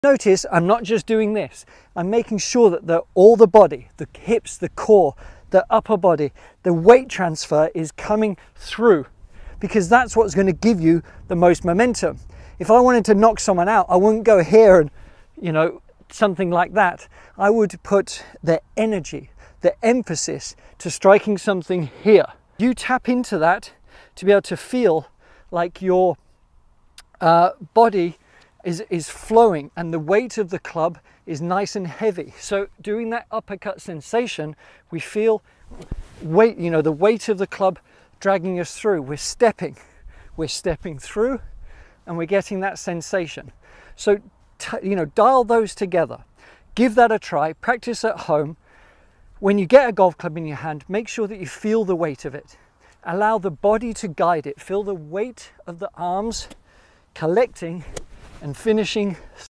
Golf Swing Punch Quick Tip For Power
Golf swing power made easy with a puch. Learn to use the ground correctly in the golf swing to generate speed and power for effortless golf swing power that you can rely on to be consistent.